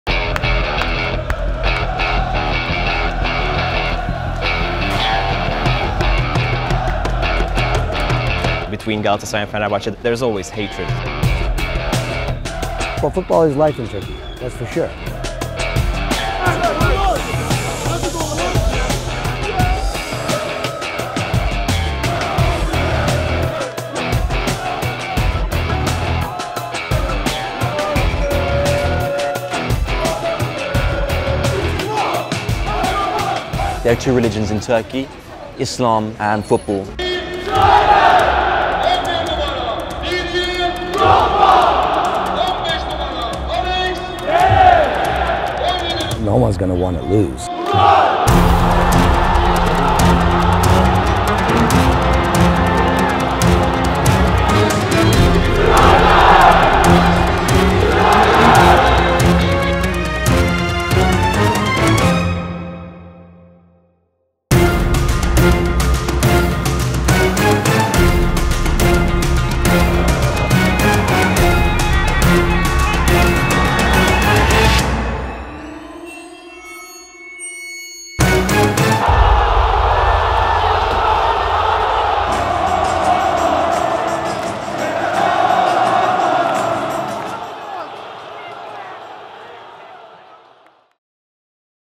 Turkey's Galatasaray and Fenerbahce Rivalry is More Insane than You Can Imagine
"There are two religions in Turkey: Islam and Football"

No one gets crazier or uses more flares in the crowd than the fans of Galatasaray-Fenerbahce rivalry